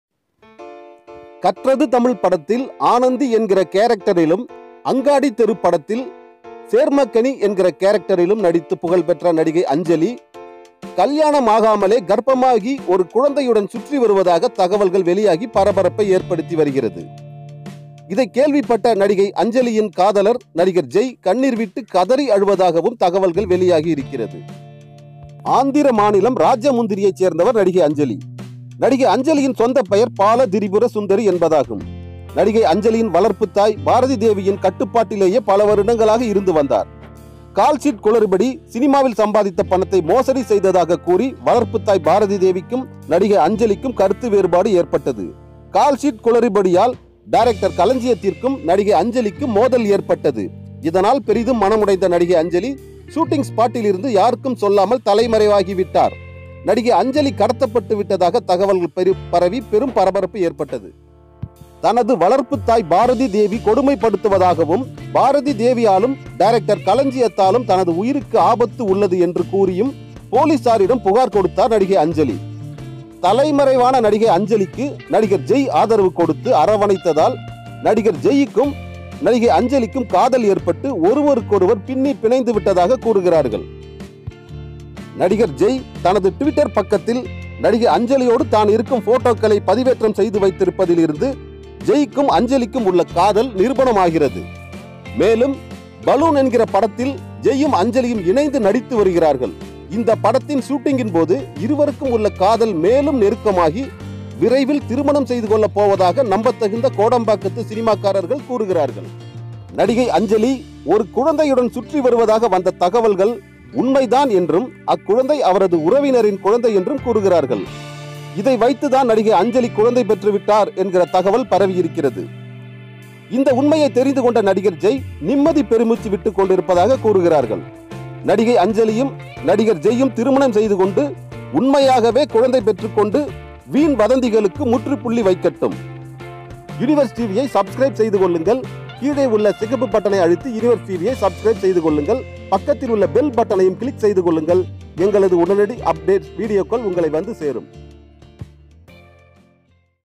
கல்யாணம் ஆகாமலே கர்ப்பமான "நடிகை அஞ்சலி!" கதறியழும் காதலன் நடிகர் ஜெய்! ACTRESS ANJALI SECRET LOV
Actress Anjali secret love with Actor Jai. Is this true? Yes... this is true... kollywood cinema people told. Because... actor jai saved actress Anjali photos in secret. But, FANS search and collected actress Anjali and actor jai jointly photos. Kollywood cinema people told Anjali and jai will marriage soon. Now... actress Anjali and actor jai acted new movie "Baloon".